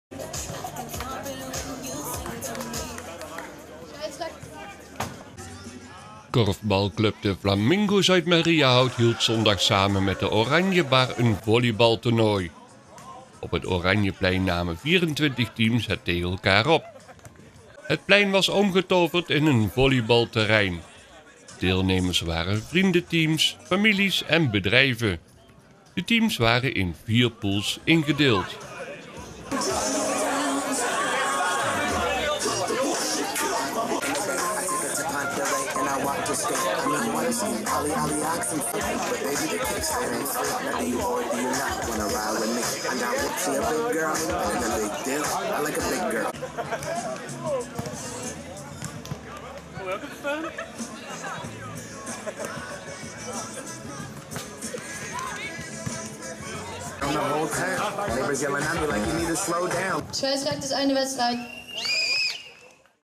2017 06 25 Volleybal tournooi MariaHout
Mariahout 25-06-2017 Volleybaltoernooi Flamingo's

Korfbalclub Flamingo's uit Mariahout hield zondag samen met de Oranjebar een volleybaltoernooi. Op het Oranjeplein namen 24 teams het tegen elkaar op. Het plein was omgetoverd in een volleybalterrein. Deelnemers waren vriendenteams, families en bedrijven. De teams waren in vier poules ingedeeld.